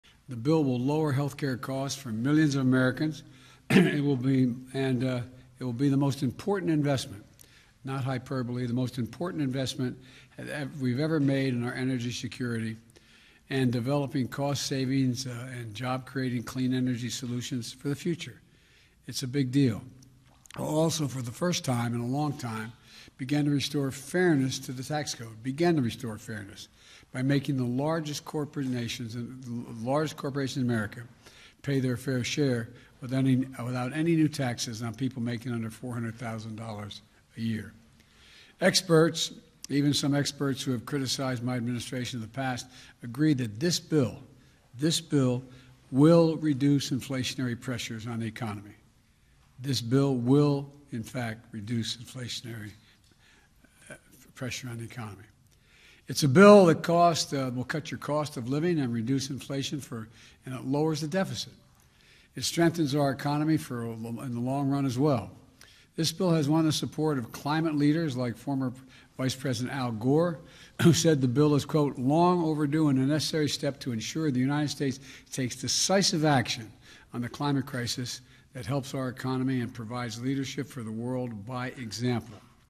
Biden: Senate Bill Will Begin to Restore Tax Fairness
President Biden says Senate Democrats’ climate-and-tax package will reduce inflationary pressures on the economy and make the tax code more fair.